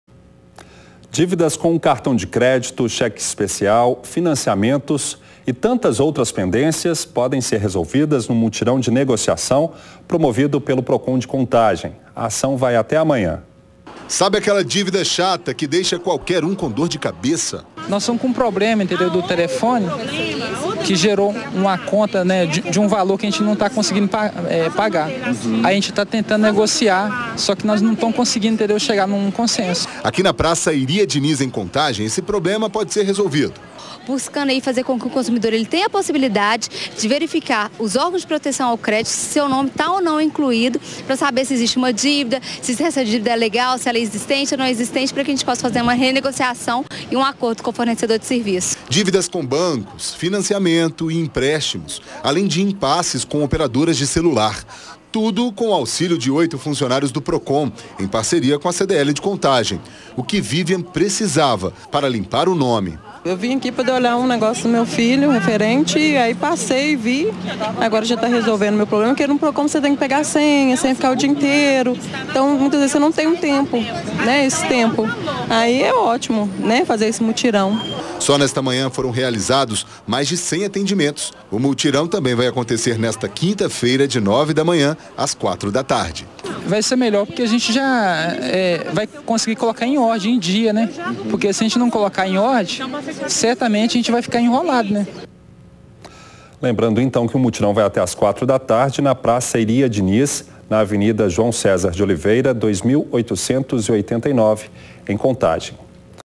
Contagem: mutirão para negociar dívidas
Para quitar essas dívidas, o Procon criou um posto que vai funcionar até amanhã na Praça Iria Diniz, na Av, João César de Oliveira, nº 2.889.